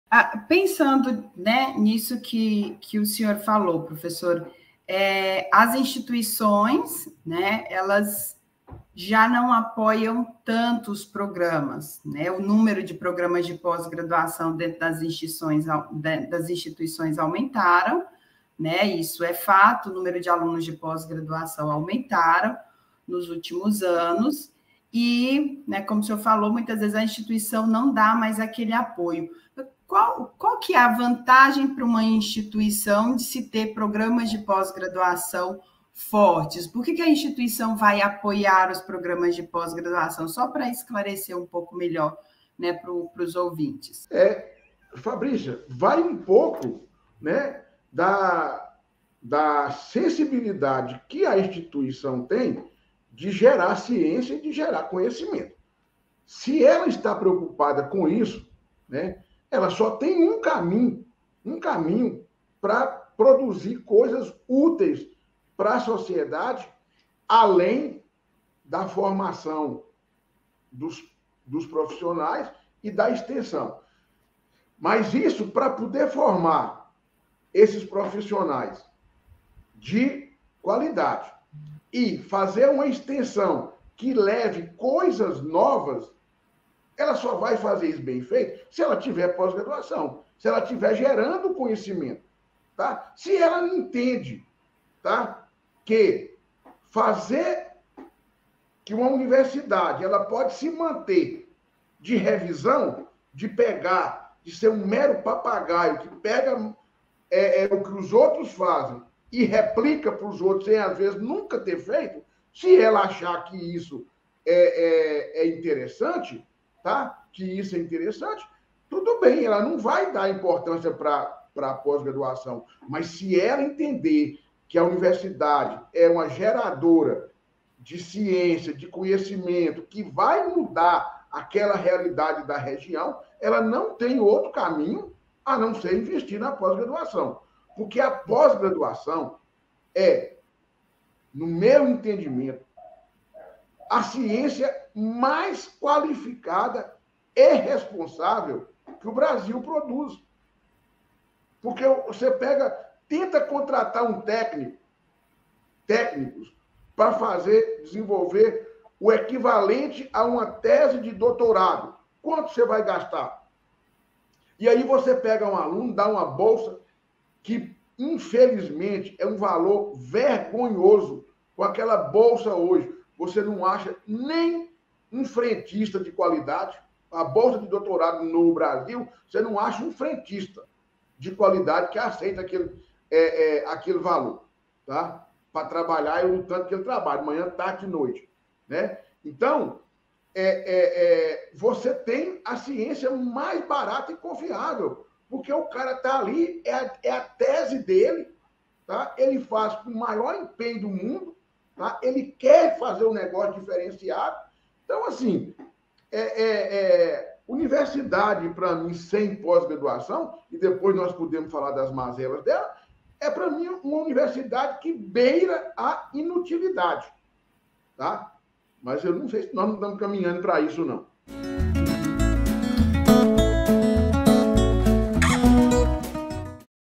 UNIVERSIDADE SEM PÓS GRADUAÇÃO É INÚTIL?
Depois de anos com o financiamento para a pós-graduação minguando, será que as Universidades brasileiras podem se tornar inúteis, por não gerarem conhecimentos?